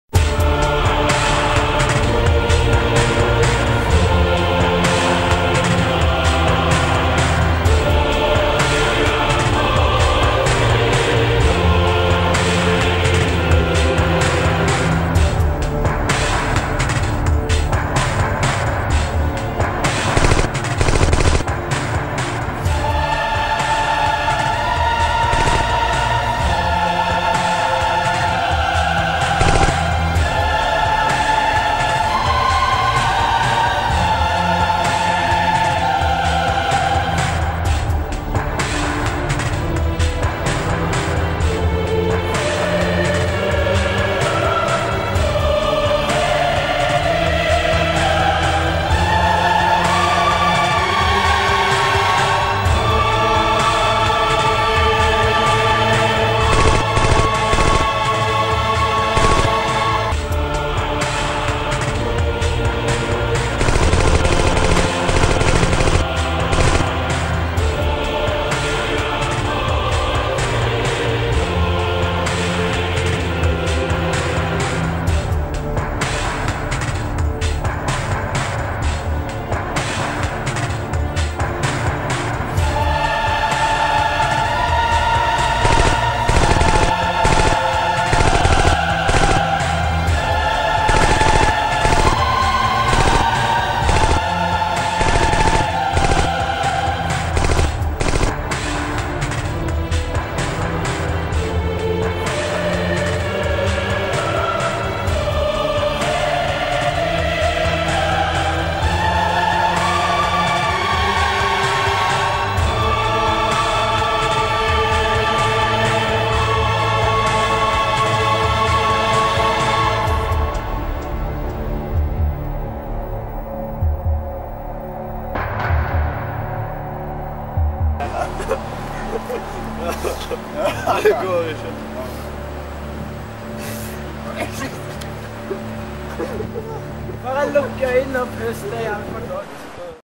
Ghost Raccoons TRAILER
The promo trailer for the norwegian airsoft- and videogame-club Ghost Raccoons.